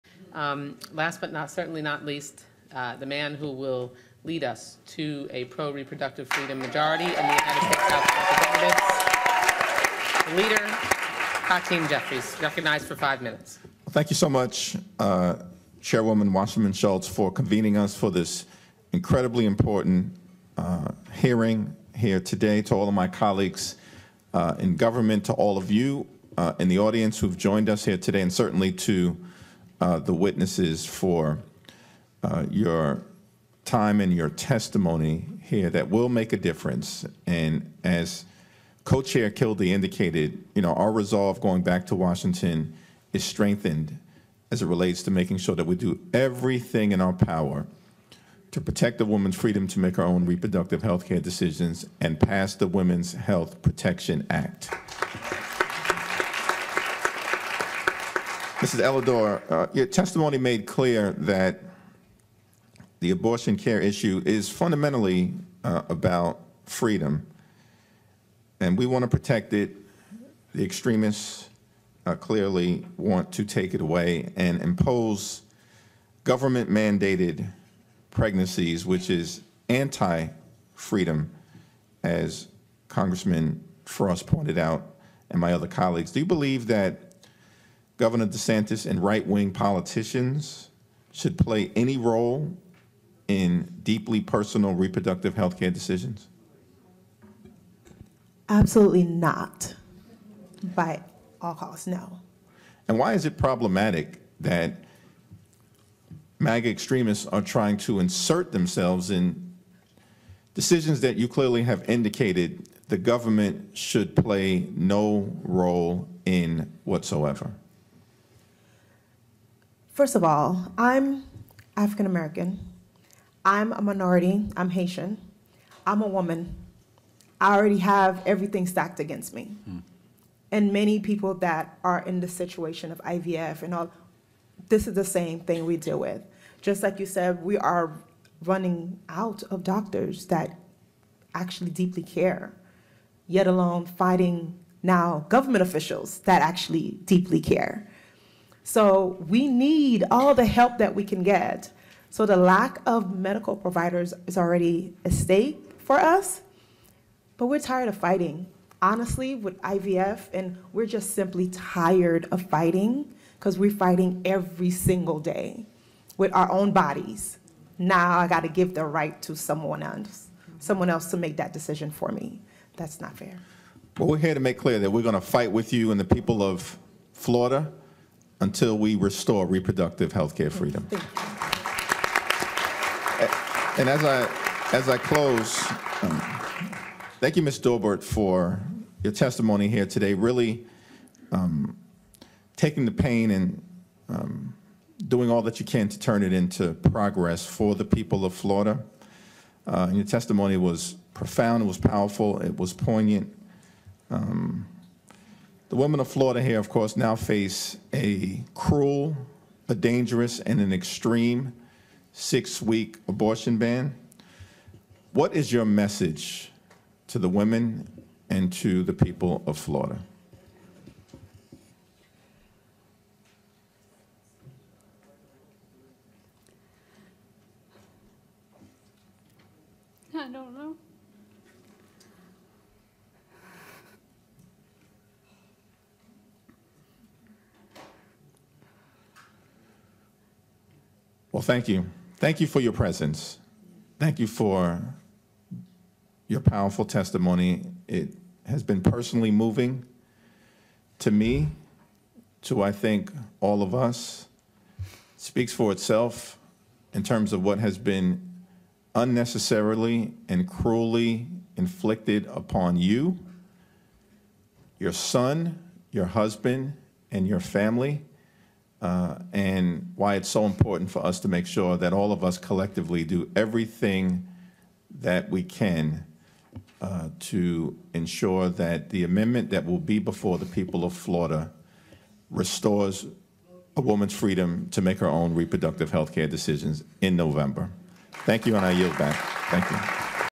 Hakeem Jeffries: Extremists Want To 'Impose Government-Mandated Pregnancies
At a House Democratic Steering & Police Committee field hearing in Florida on Tuesday, Rep. Hakeem Jeffries (D-NY) spoke about reproductive freedom.